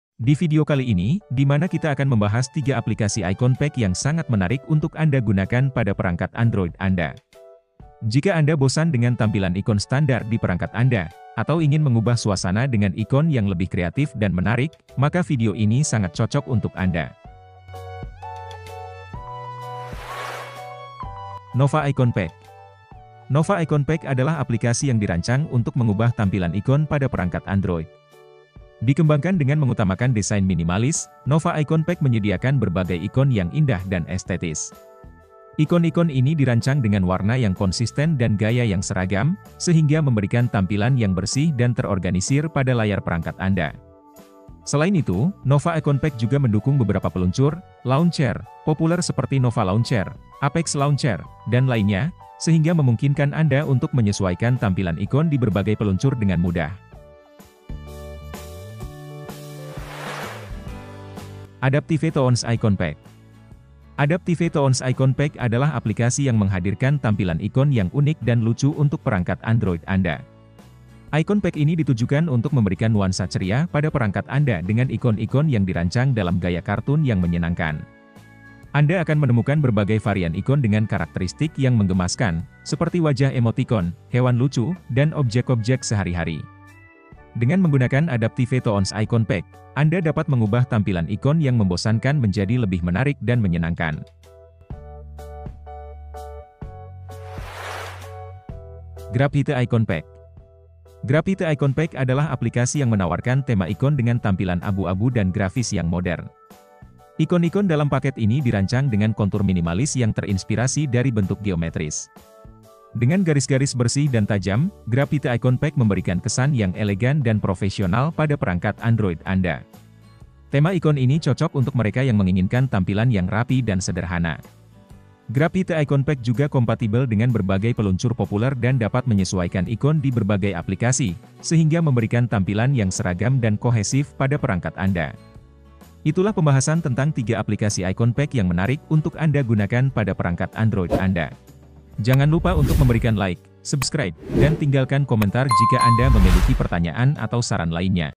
3 Aplikasi Icon Pack Untuk Android Terbaik
Dalam video ini, kami akan mengulas dan merekomendasikan beberapa aplikasi icon pack terbaik Android untuk membuat tampilan smartphone Anda terlihat lebih keren dan serasi. Icon pack merupakan sebuah aplikasi yang memungkinkan Anda mengubah tampilan ikon-ikon pada layar smartphone Anda menjadi lebih elegan dan menarik. Kami akan membahas beberapa icon pack keren yang bisa Anda gunakan untuk mengubah tampilan smartphone Anda menjadi lebih menarik dan unik.

Tidak hanya itu, kami juga akan memberikan penjelasan mengenai tampilan dan fitur-fitur yang dimiliki icon pack terbaik yang kami rekomendasikan. Dengan menggunakan aplikasi icon pack terbaik, Anda dapat menjadikan smartphone Anda menjadi lebih keren dan berbeda dari yang lain. Icon pack terbaik dan keren akan membuat pengalaman menggunakan smartphone Anda menjadi lebih menyenangkan dan lebih memuaskan.